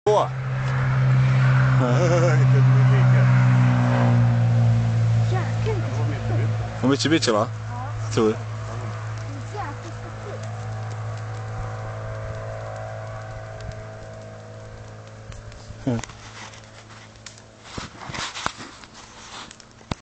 En Mitsubishi Rallybil kör förbi när vi ska grilla. 2010.
Odensala, Uppland, Sverige. En ganska rakrörad Mitsubishi med relativt låg utväxling kör förbi.